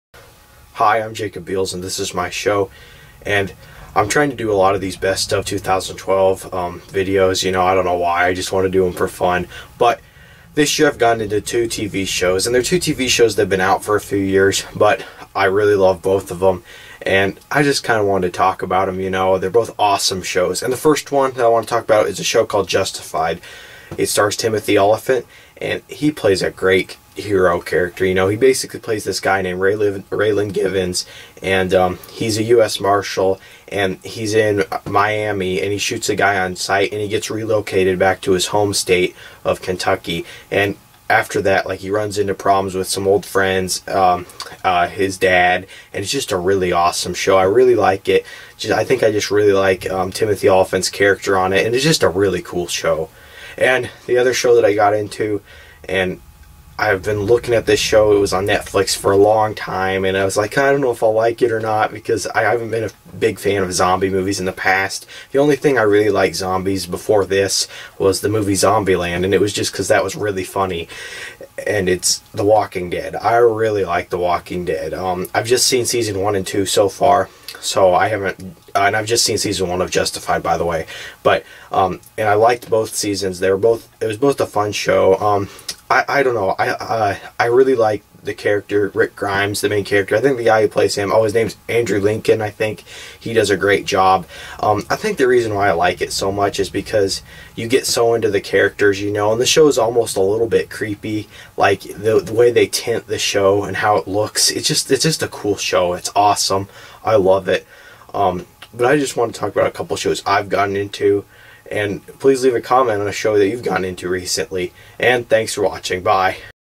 Two Tv Shows I've Gotten Into in 2012!
After a few years I finally picked up on two great shows!
1. Justified
2.The Walking Dead